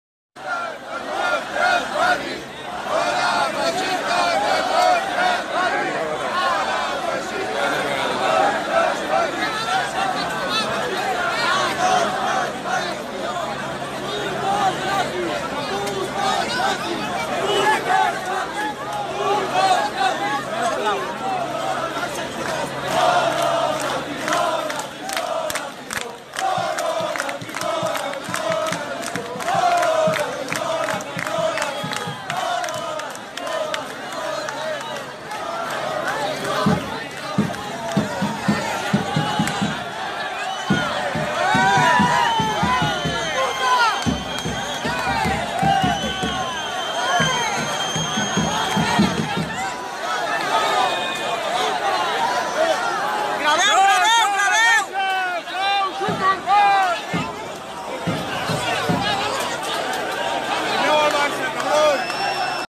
Altercados en los aledaños del Camp Nou: 46 heridos
Un grupo de unos 30 ultras e independentistas convocados por Tsunami Democràtic se han enfrentado a golpes en los aledaños del Camp Nou, sobre las 19.45 horas.